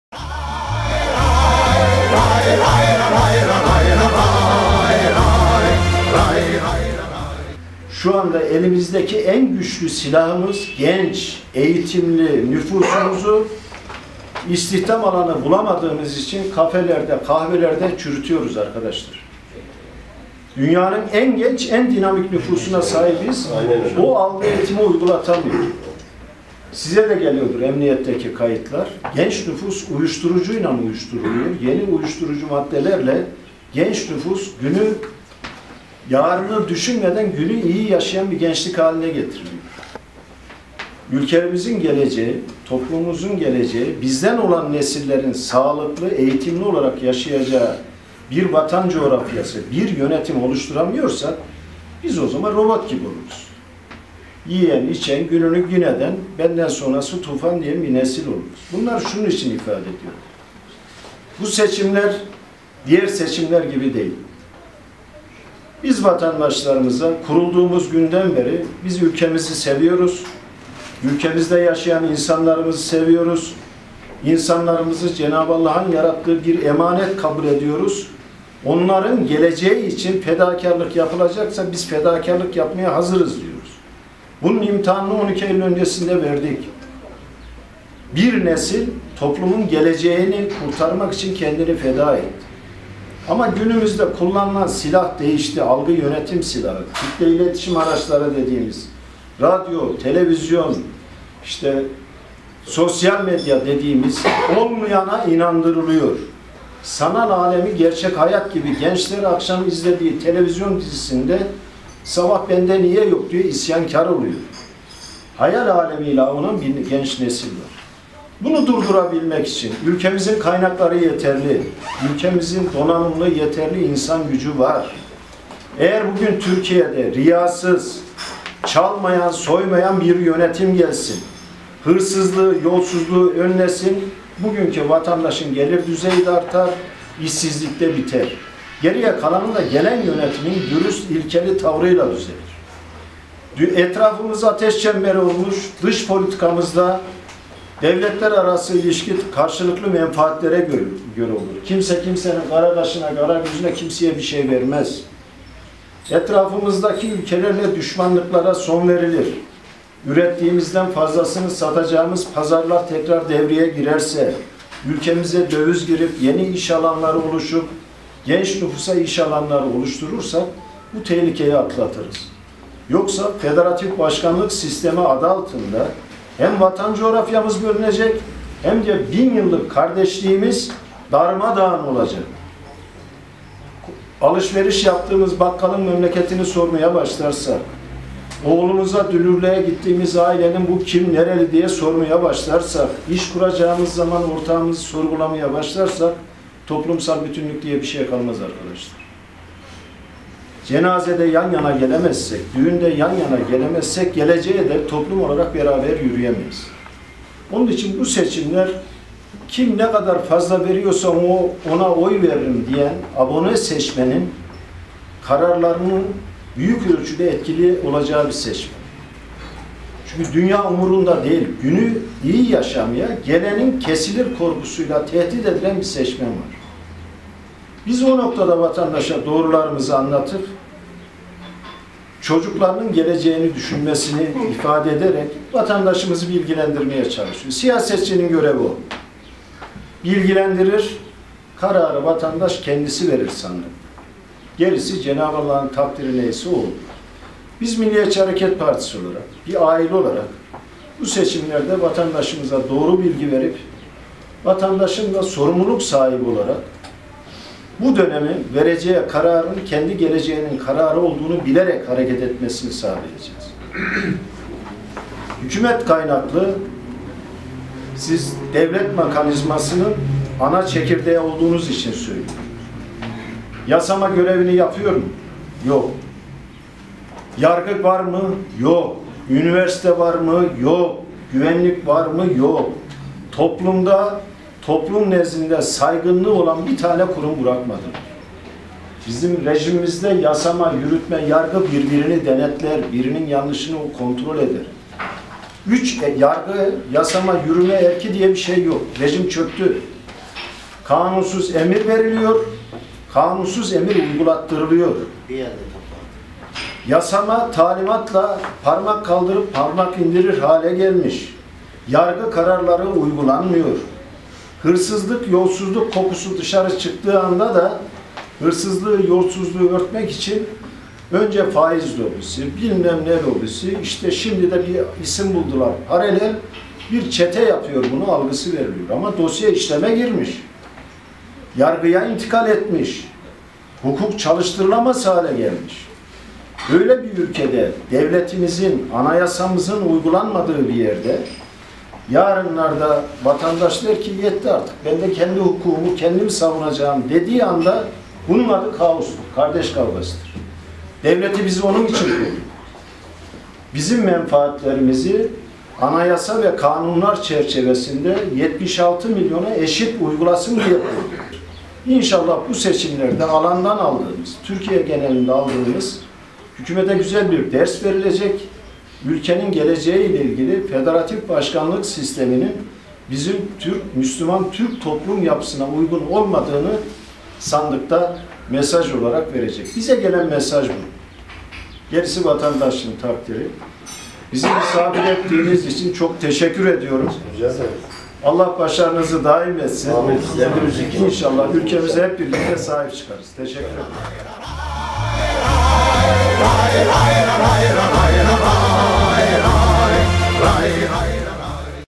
Şefkat Çetin Keçiören Muhtarlar Derneği konuşması
MHP Teşkilat İşlerinden Sorumlu Genel Başkan Yardımcısı Şefkat Çetin'in, Keçiören Muhtarlar Derneği'nde yapmış olduğu konuşma